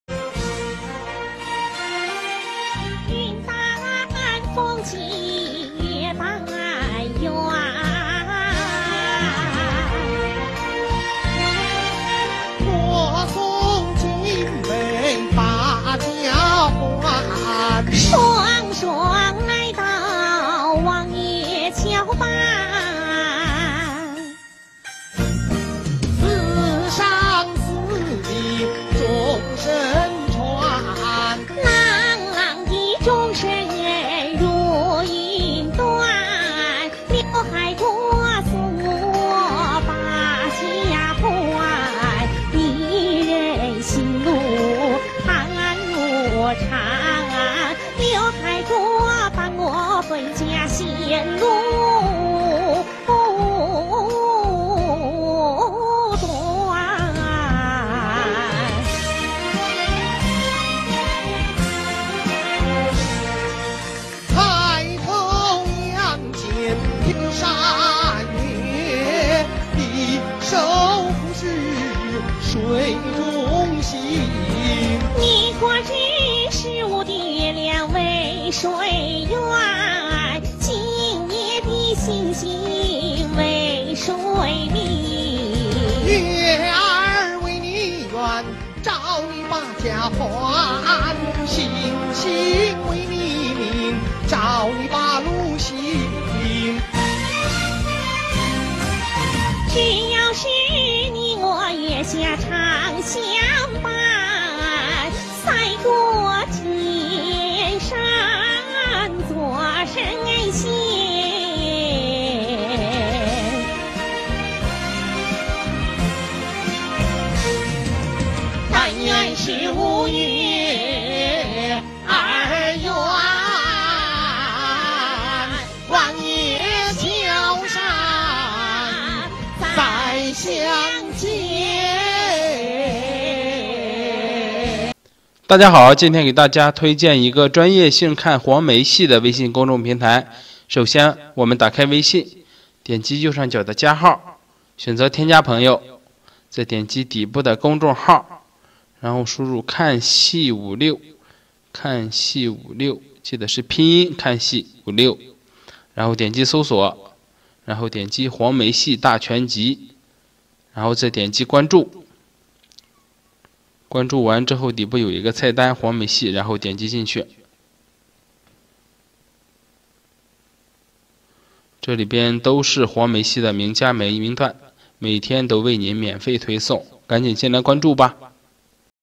父女同台唱黃梅戲，韻味十足！ 【曲藝薈萃】
父女同台唱黄梅戏，韵味十足！ | Father and daughter sing Huangmei opera together, full of charm!